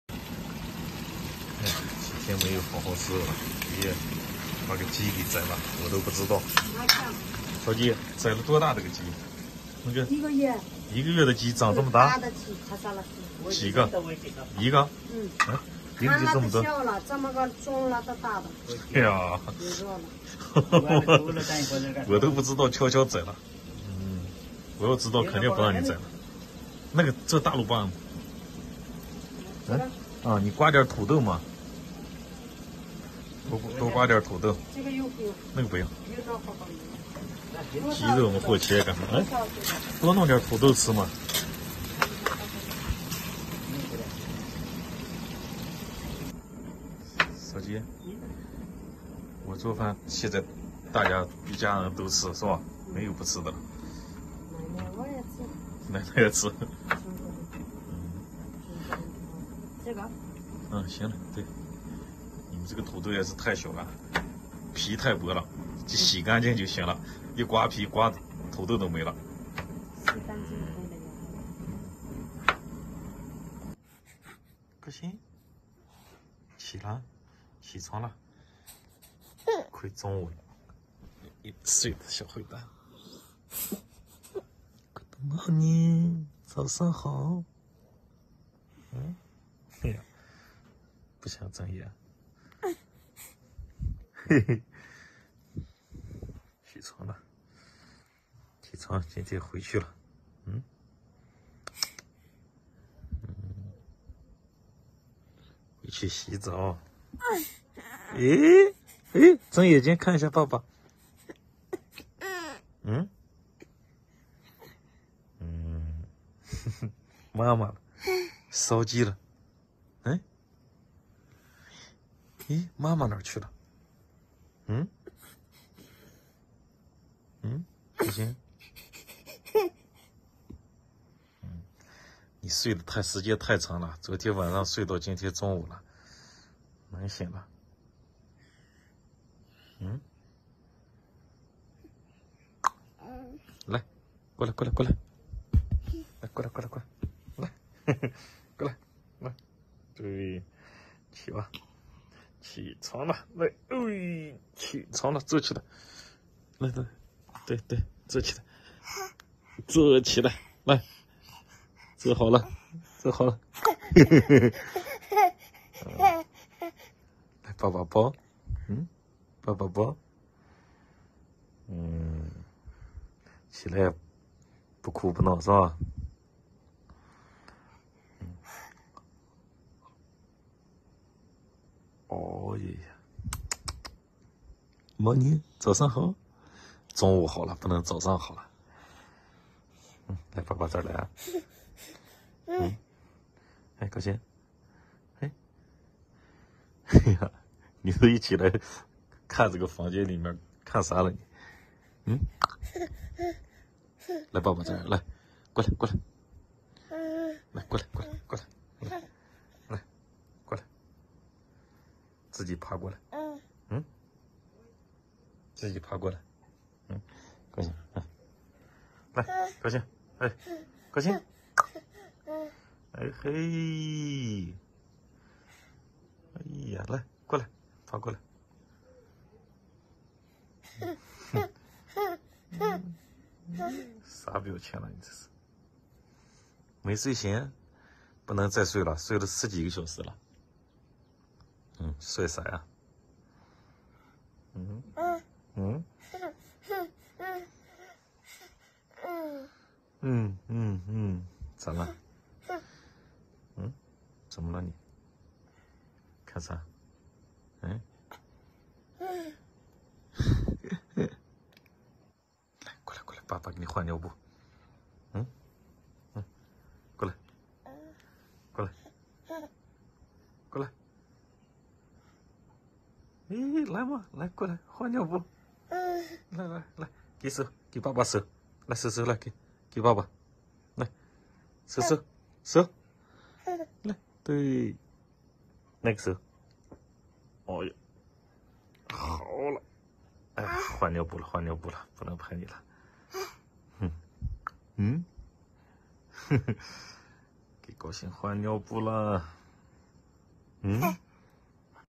爷爷怕把烧鸡饿坏了，偷偷宰了一只鸡，高兴起床也是不哭不闹【尼泊尔高哥幸福的一家】
这里是尼泊尔高哥幸福的一家